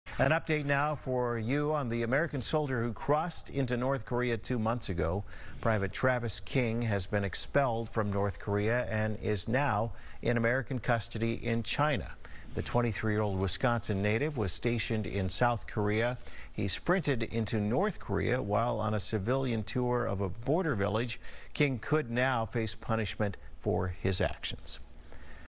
U.S. solider expelled from North Korea
The American solider who crossed into North Korea is now in U.S custody. Private Travis King ran across the border while on a civilian tour of a South Korea border town.